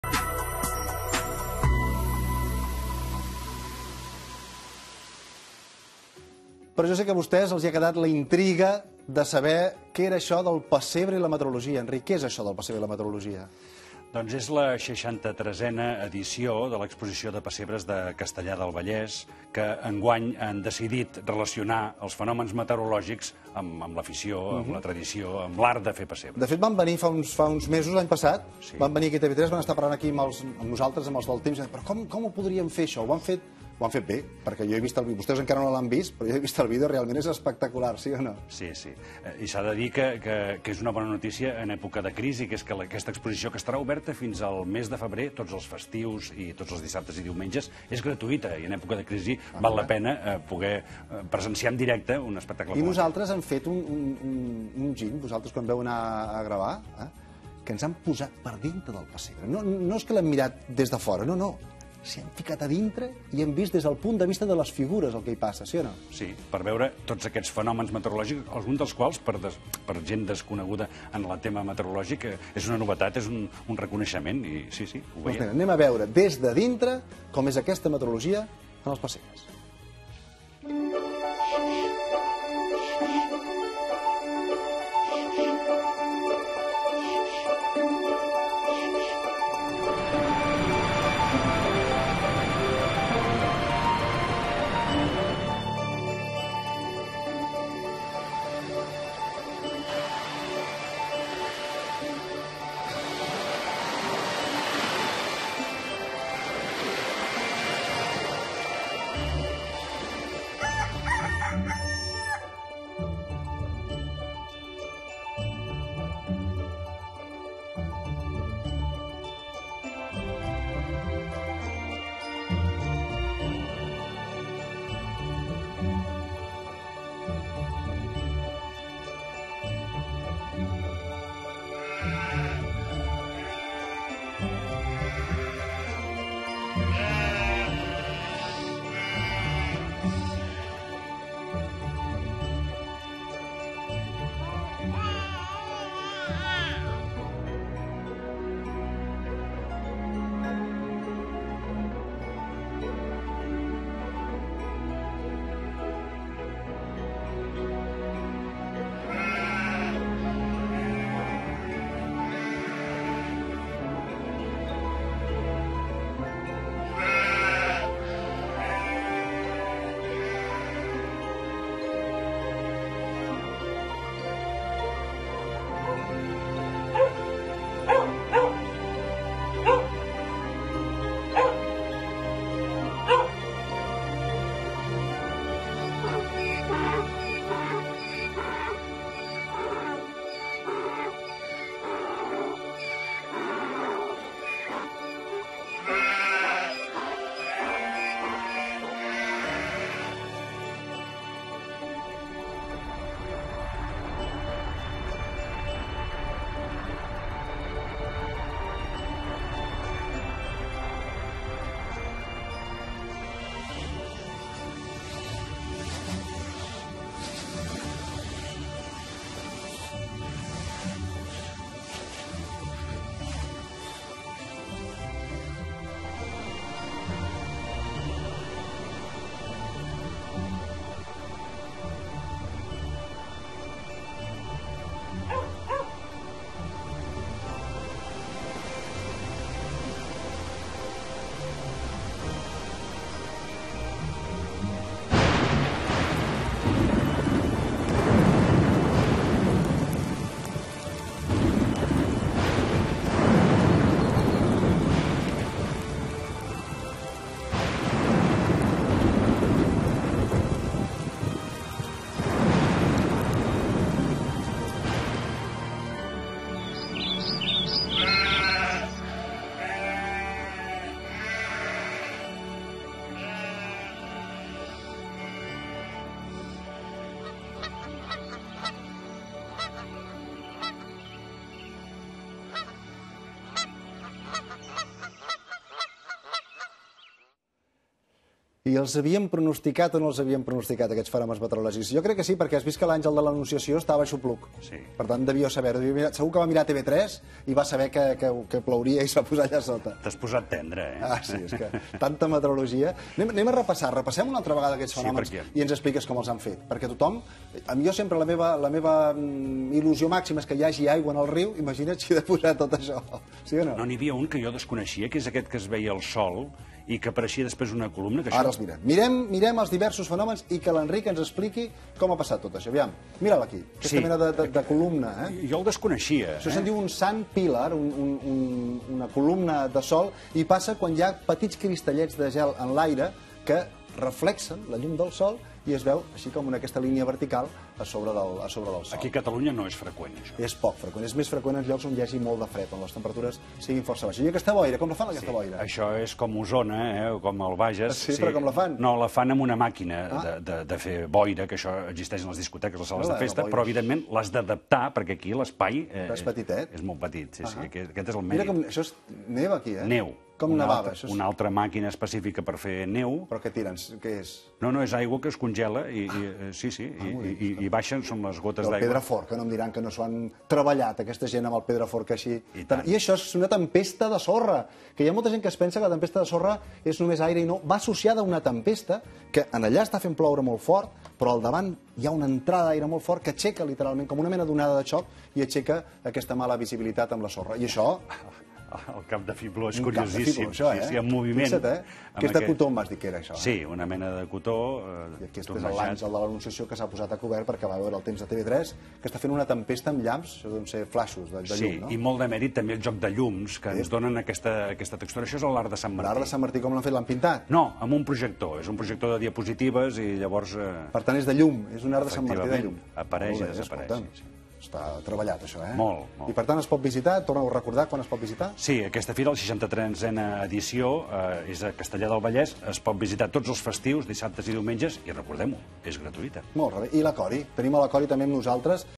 Espai Terra Pessebres 2013
El 30 de novembre passat es va inaugurar la 63ª Exposició de Pessebres de Castellar del Vallès. Els diorames estaran exposats fins el 2 de febrer del 2014, diumenges de 12h. a 14h. i de 17 a 20h.
Enguany, l'exposició te com a eix temàtic els fenòmens meteorològics, un repte tècnic diferent pels membres de l'associació, que han esdevingut tot un referent del sector a nivell de Catalunya.
Com veurem, a cadascun dels diorames dels 17 pessebres de l'exposició "El Cel i el Temps" hi podrem observar un fenòmen diferent.: Boira, nevada, glaçada, dia de sol, pluja, arc de sant Martí, tormenta d'arena , pluja d'estels, caps de fibló
Un equip de l'Espai-Terra ha visitat les instal.lacions per portar-nos els detalls més divertits i curiosos d'aquesta excepcional exposició .